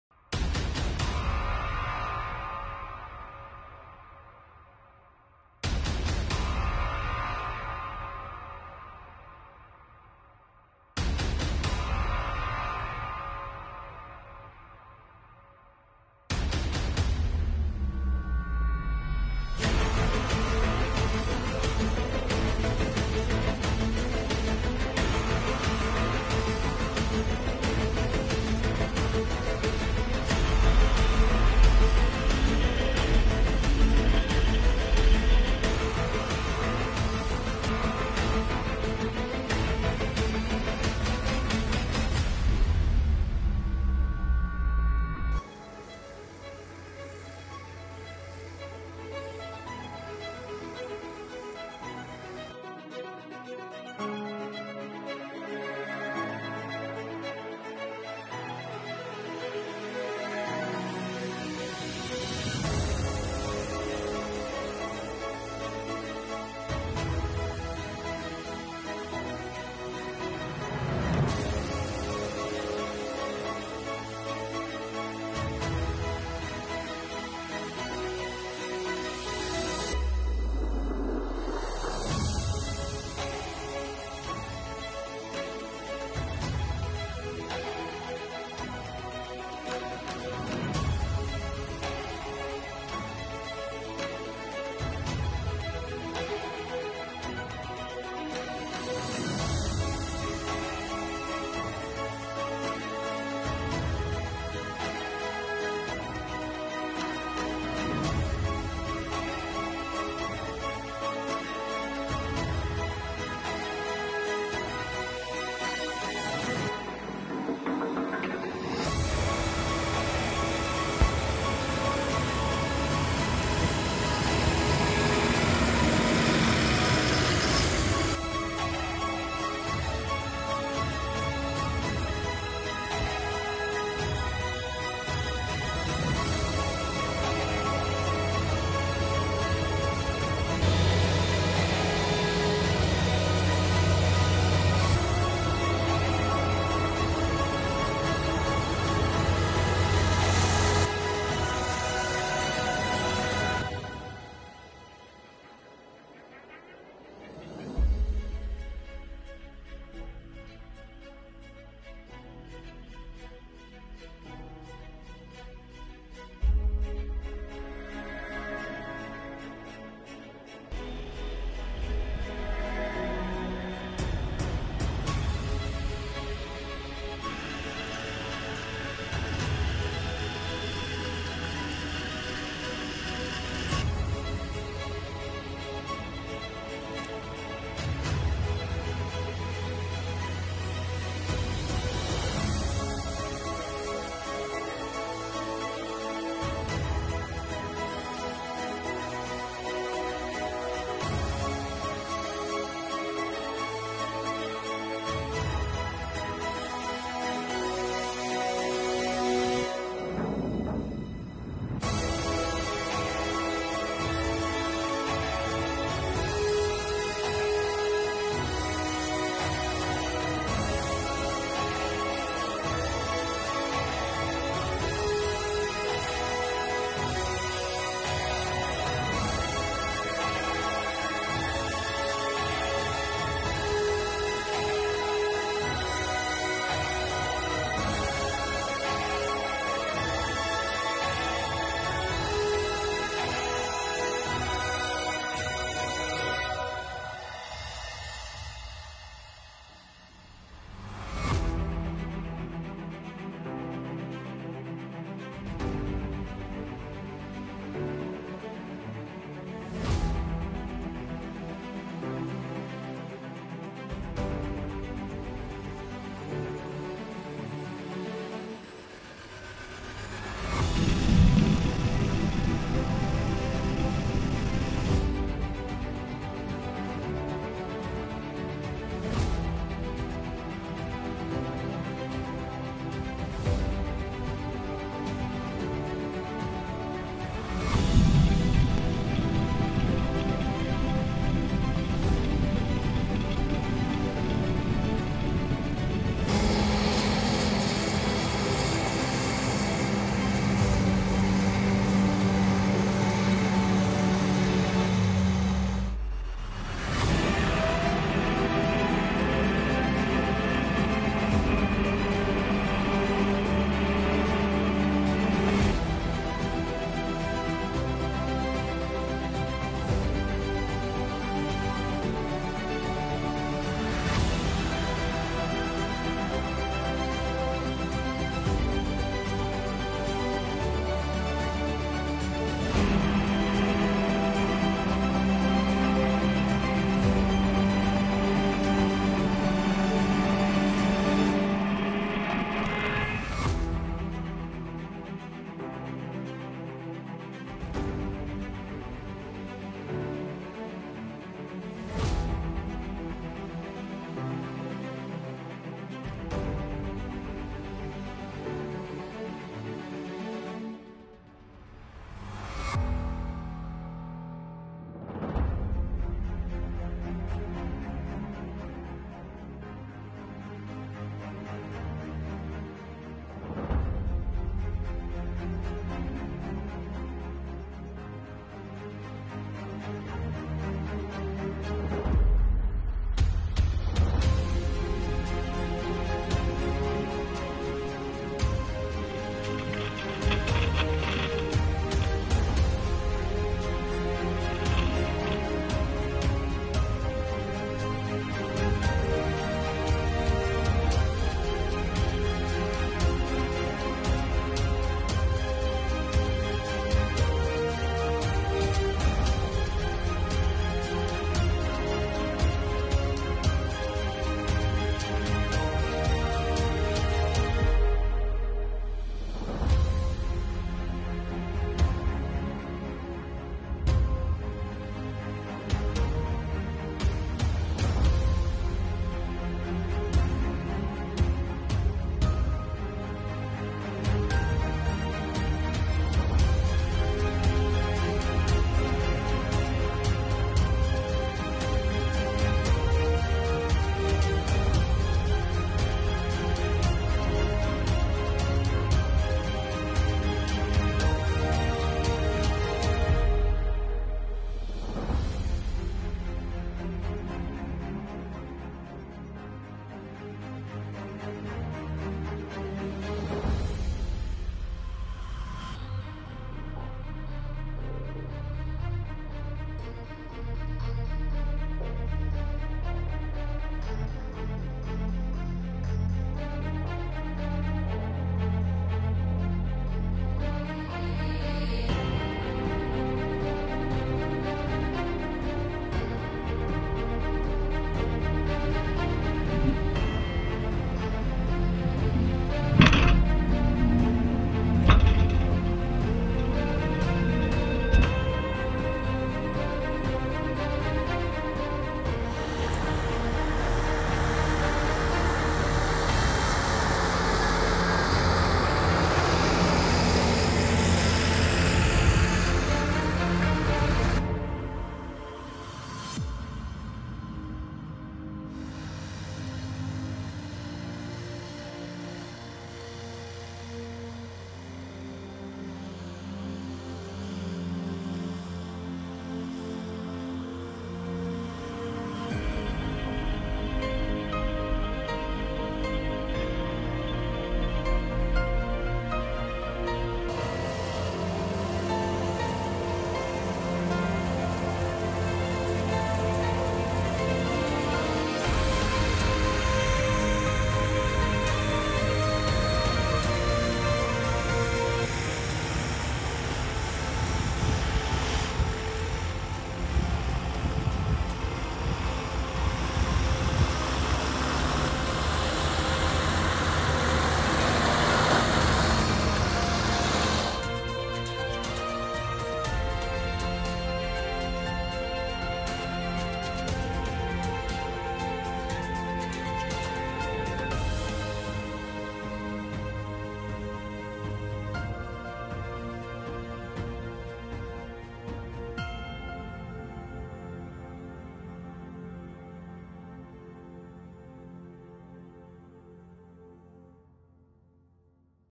Maishäckseln 2020| Fendt, John Deere, Claas
Maishäckseln 2020 auf dem Hirschweidenhof.